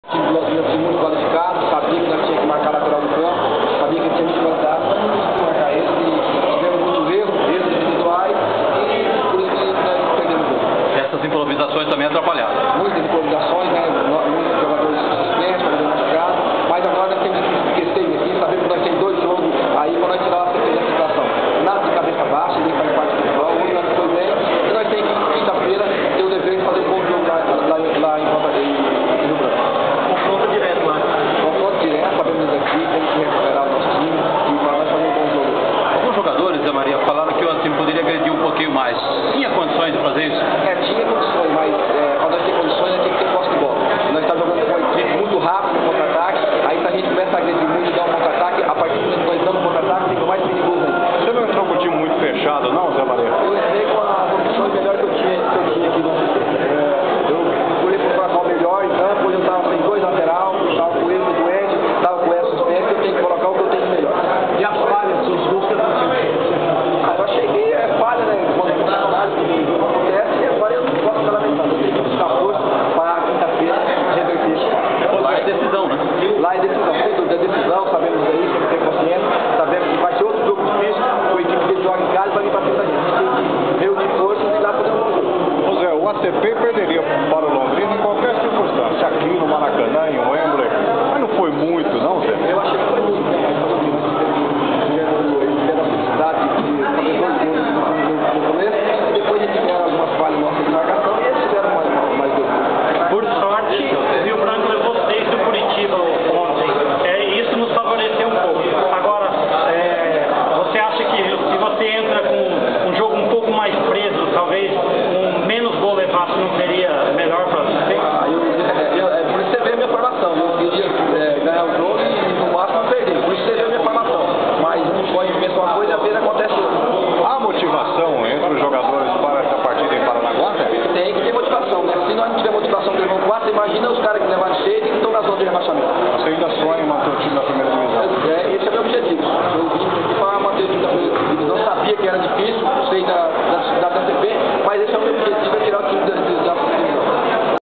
Coletiva com o Técnico José Maria Barbosa após a derrota para o LEC por 04 x 00
O ACP com algumas improvisações tentou segurar o Londrina, mas não conseguiu, mas mesmo perdendo por 04 x 00, o ACP saiu da zona de rebaixamento, invertendo a posição com Rio Branco que perdeu na abertura da 9ª rodada para o Coritiba por 06 x 00. No critério de desempate de saldo de gols o ACP em dois gols a mais.